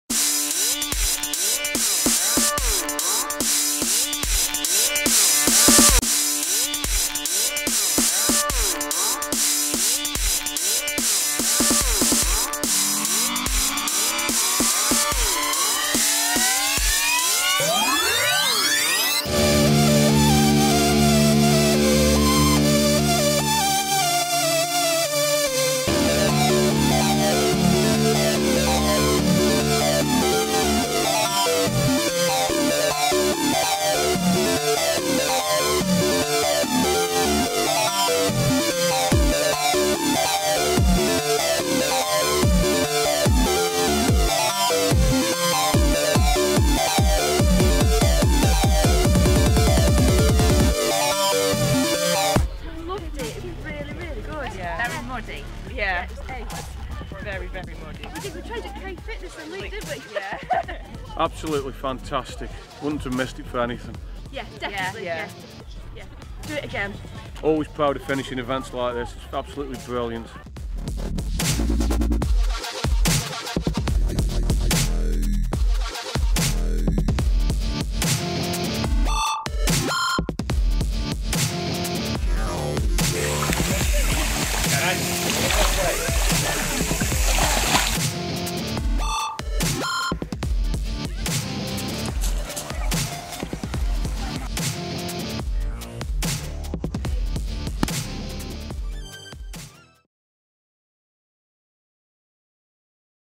Extreme Stampede 2016
Extreme Stampede takes place at Uttoxeter Racecourse on Sunday 23rd October 2016. The 10k race will put participants through their paces as they climb over hurdle fences, run through water and face the challenge of the BIG ONE.

If you’re not up for the full 10k, this year you can run 5k instead! So, whether this is your first race or you are a competitive runner, the Extreme Stampede is a race for everyone.